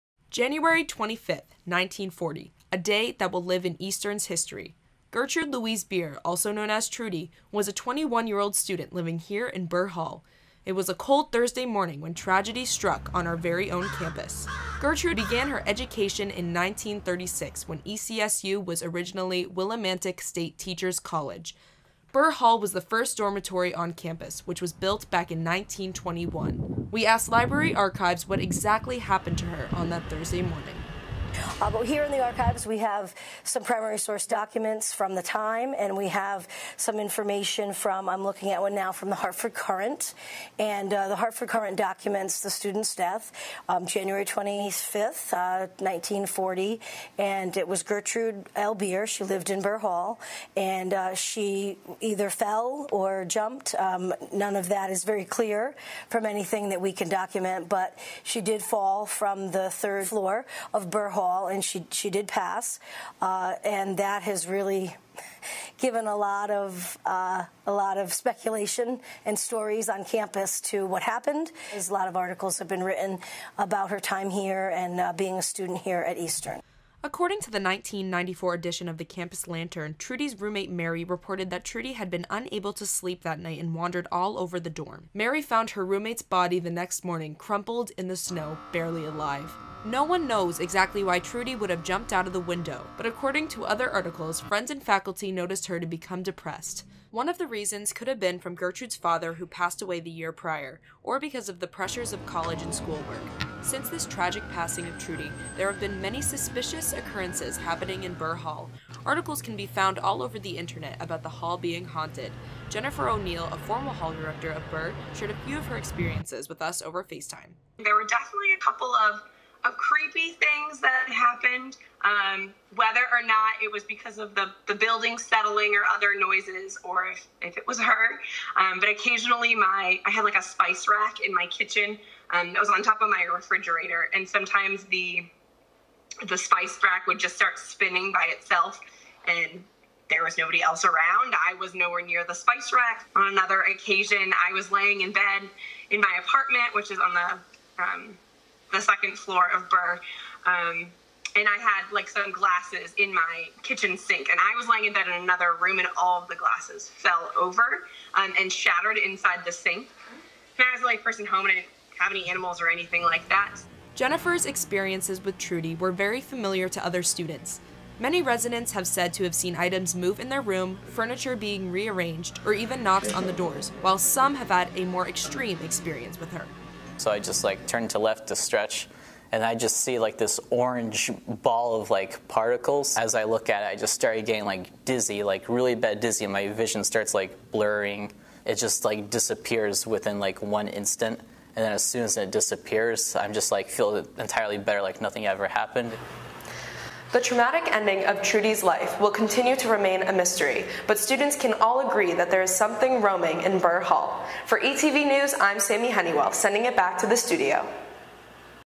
Burr Hall- ETV News
Sammy Honeywell reports on the true story of the tragic passing of Gertrude Louise Beer on Eastern Connecticut State University's very own campus. This occurred in 1940, but residents still feel her presence in Burr Hall.